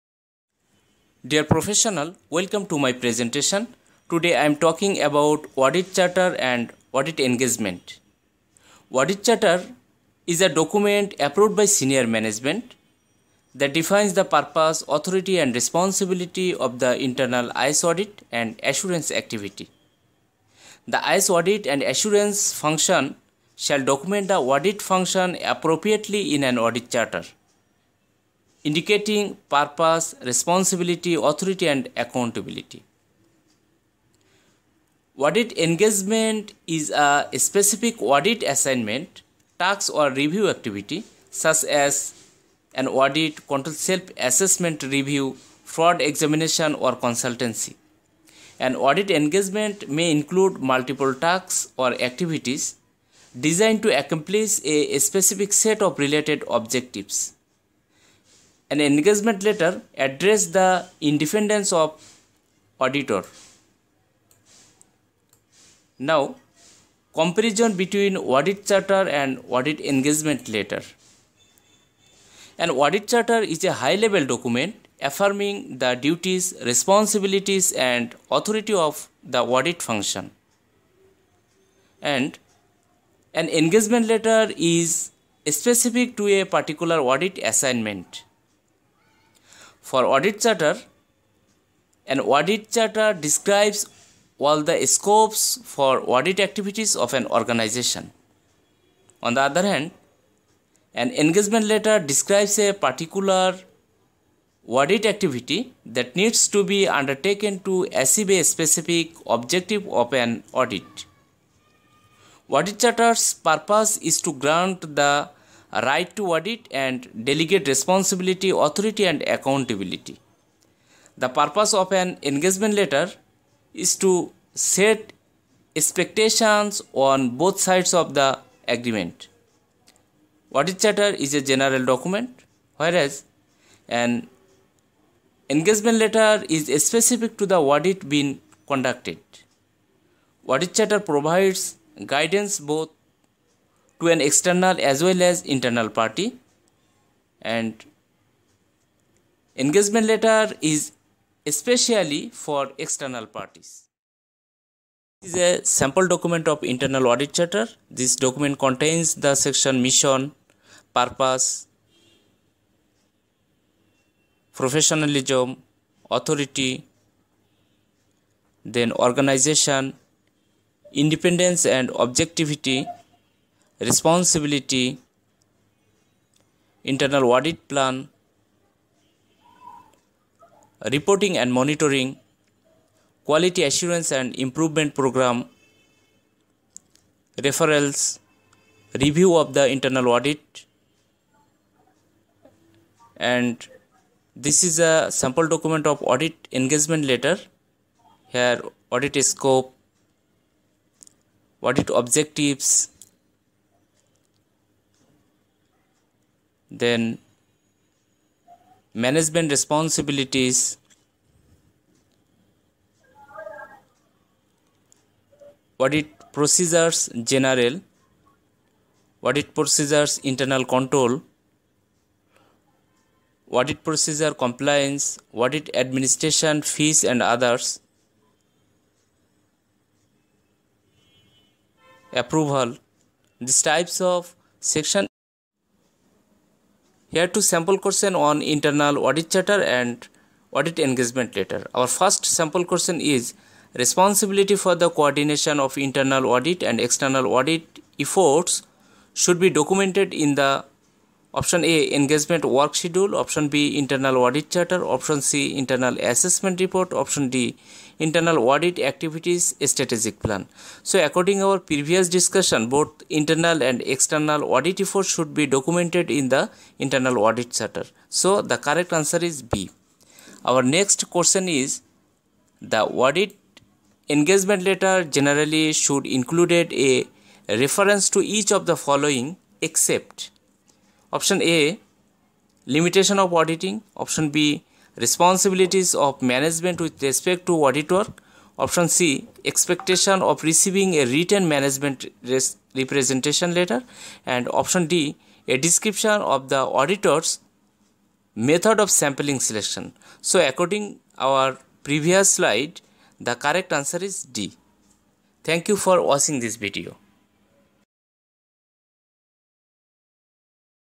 Audit Charter & Audit Engagement | Audit Charter vs Audit Engagement Letter
Q-1: Responsibility for the coordination of internal audit and
external audit efforts should be documented in the:
A. Engagement work schedule
B. Internal audit charter
C. Internal assessment report
D. Internal audit activity’s strategic plan

Q-2: The audit engagement letter, generally, should include a reference
to each of the following except:
A. Limitations of auditing
B. Responsibilities of management with respect to audit work
C. Expectation of receiving a written management representation letter.
D. A description of the auditor’s method of sample selection.

Q-3: An audit charter should:
A. be dynamic and change often to coincide with the changing nature of technology and the audit profession.
B. clearly state audit objectives for, and the delegation of, authority to the maintenance and review of internal controls.
C. document the audit procedures designed to achieve the planned audit objectives.
D. outline the overall authority, scope and responsibilities of the audit function.

Q-4: An organization's IS audit charter should specify the:
A. short- and long-term plans for IS audit engagements
B. objectives and scope of IS audit engagements.
C. detailed training plan for the IS audit staff.
D. role of the IS audit function.

Key Points:
1. An audit charter describes all the scopes of audit activities of an organization, whereas an engagement letter describes a particular audit activity that needs to be undertaken to achieve a specific objective of an Audit.
2. The role of IS internal audit function should be established by an audit charter approved by senior management.
3. The engagement letter is used with independent auditors to define the relationship. This letter serves as a record to document the understanding and agreement between the audit committee and the independent auditor. It provides the independent auditor the responsibility, accountability, and authority to conduct the audit.